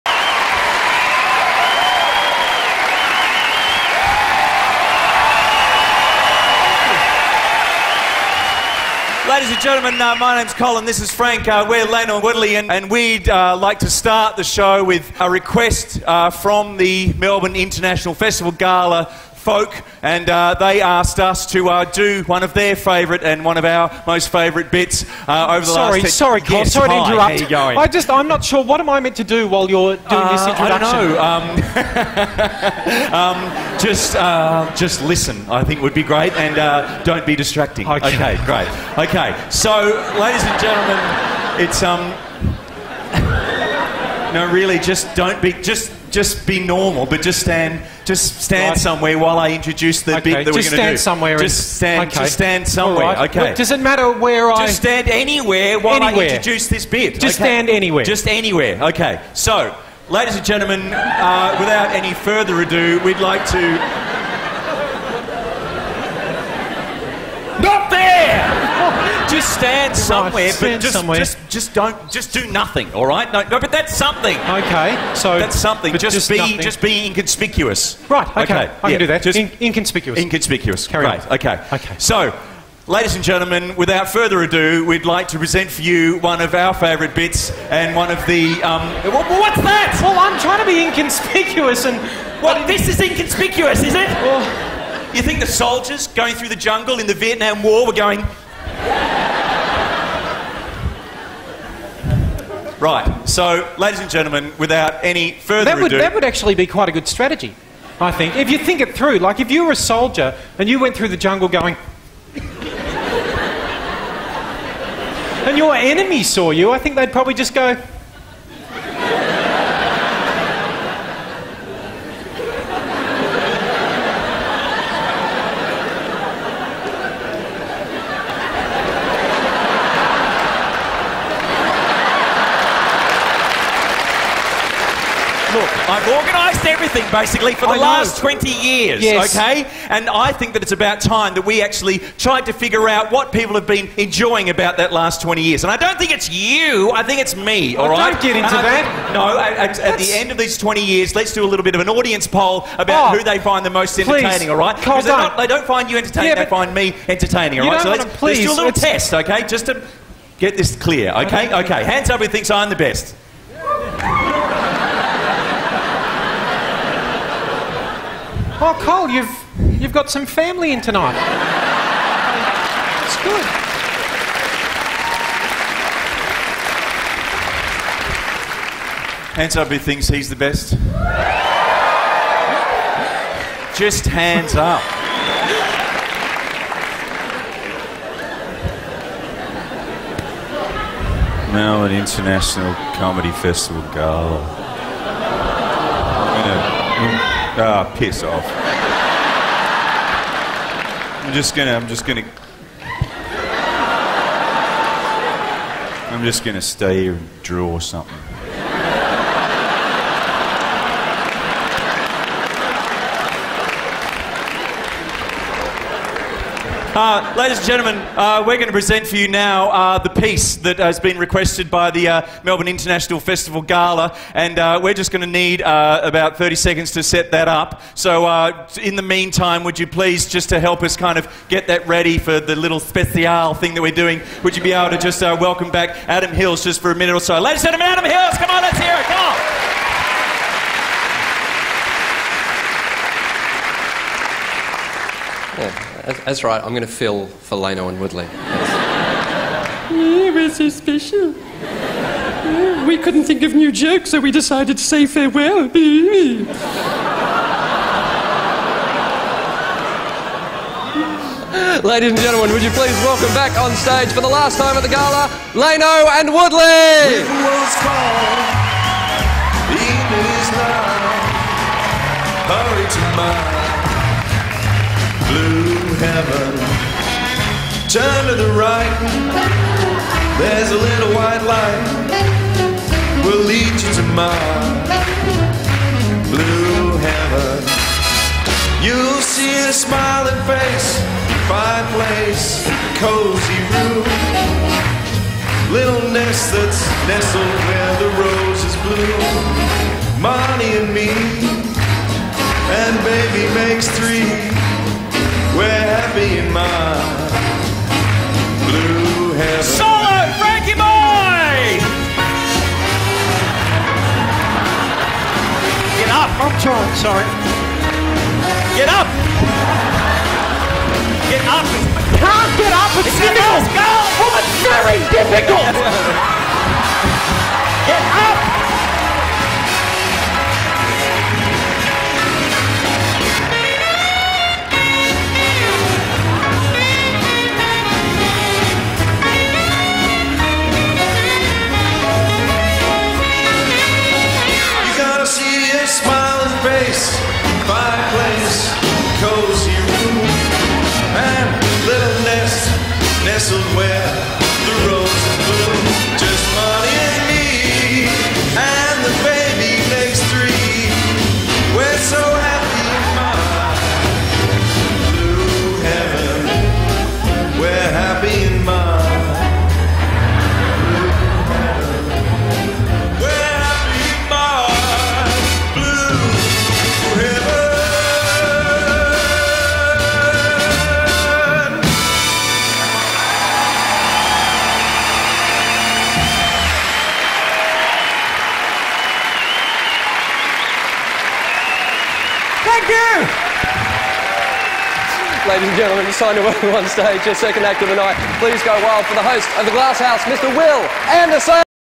Lano and Woodley - Last Performance / Sleeping bag dance
The last performance of Lano and Woodley at the Melb. Comedy Festival 2006. Very cute ^^